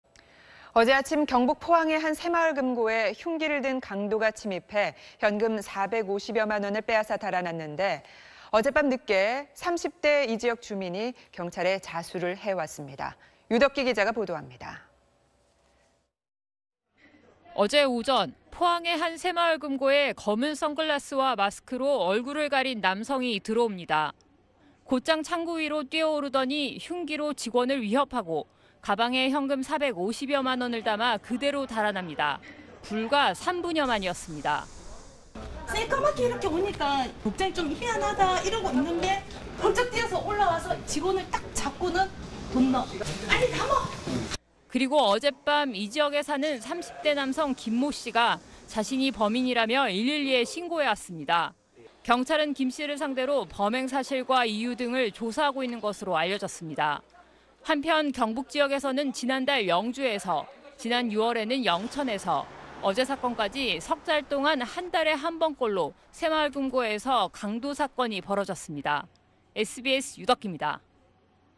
포항 새마을금고 강도 용의자 간밤에 자수…30대 남성 / SBS
어제(7일) 아침 경북 포항의 한 새마을 금고에 흉기를 든 강도가 침입해 현금 450여만 원을 빼앗아 달아났는데 어젯밤 늦게 30대 지역 주민이 경찰에 자수를 해왔습니다.